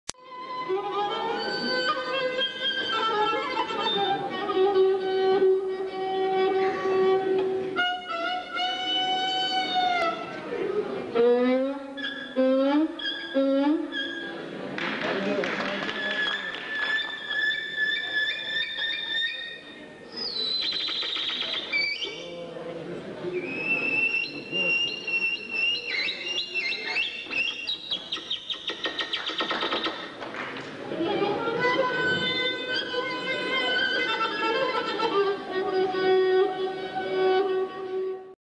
Гъдулар имитатор в Гълъбово - 13.05.2000 г.
Десети национален събор на гъдуларите в България.